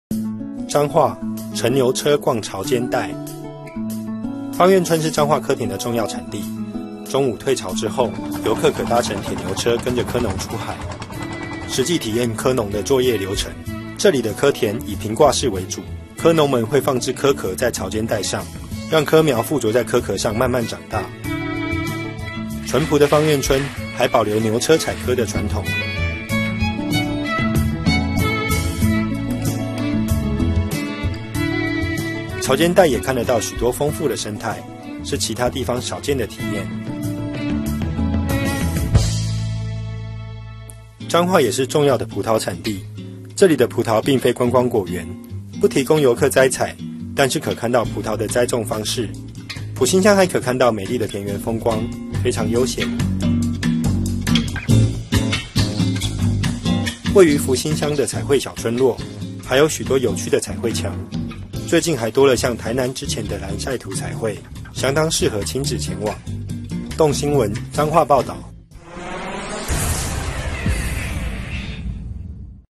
彰化搭牛車樂採蚵 遊逛彩繪村落--蘋果日報 20140618
來到盛產葡萄的彰化，在埔心鄉逛葡萄園時，行經田間小路，感受悠閒放鬆的心情；再跟著黃牛車往海邊走去，隨著芳苑村的蚵農採蚵，最後來到福興鄉的彩繪村落，還可看到近百幅創意又樸實的彩繪牆面。一趟彰化行，可感受多元的農村魅力。
報導╱許維豪 攝影╱薛泰安